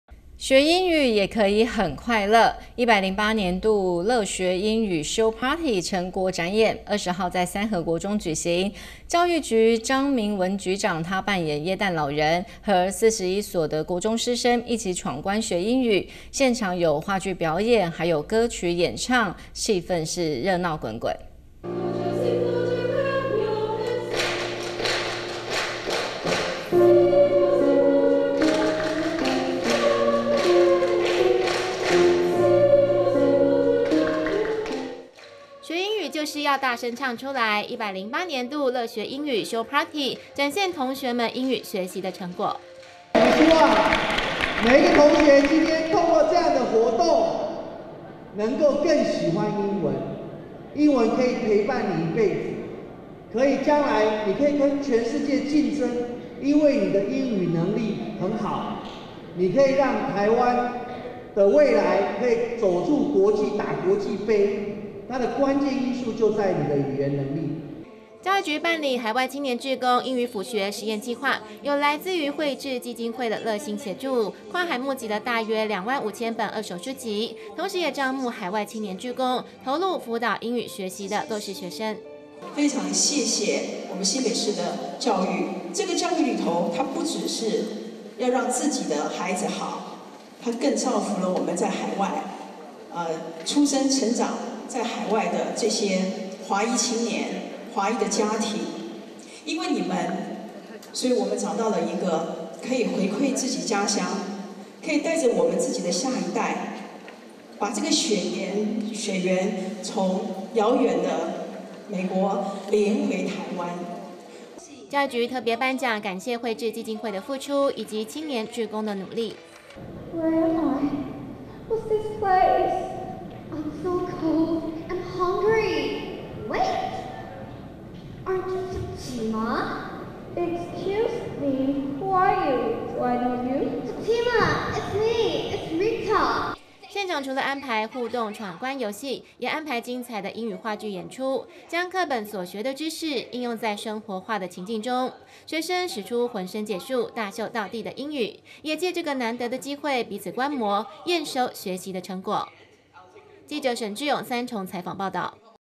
天外天新聞1081224 13樂學英語Show Party 互動闖關秀成果
學英語也可以很快樂，「108年度樂學英語Show Party」成果展演，20號在三和國中舉行，教育局張明文局長扮演耶誕老人，和41所國中師生一起闖關學英語，
現場有話劇表演、歌曲演唱，熱鬧滾滾。